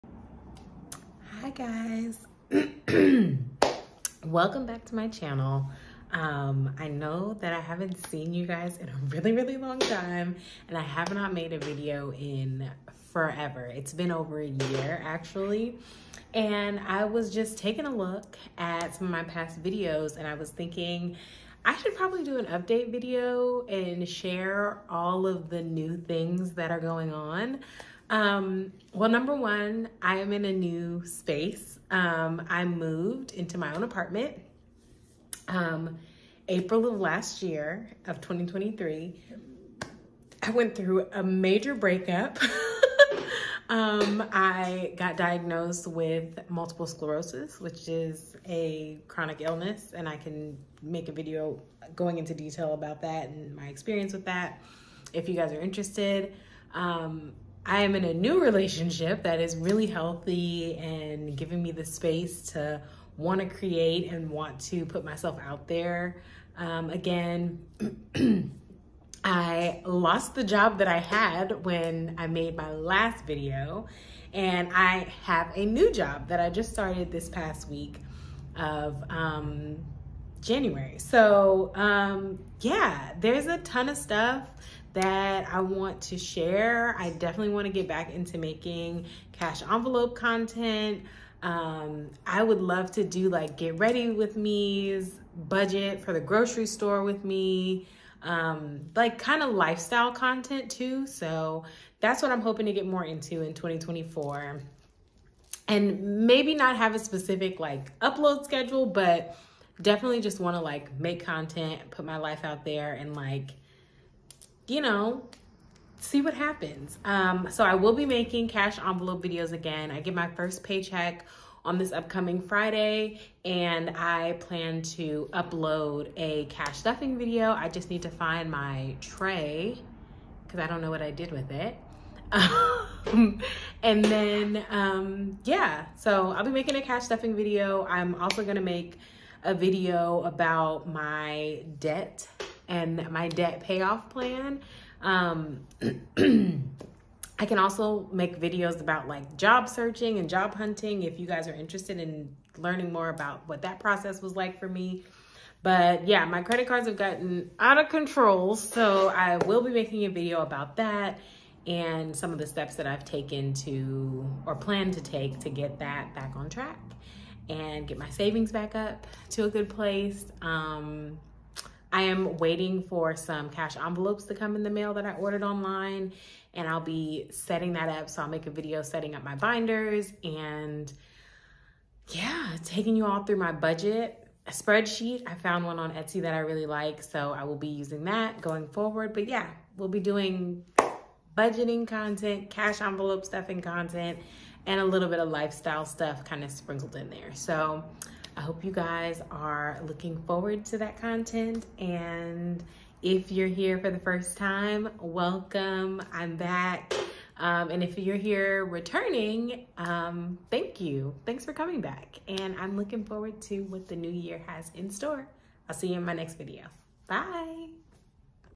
Life Update: 2024 Content
I have been gone for so long and finally ready to get back to making content.  Come along on my journey and see what the new year has in store for us!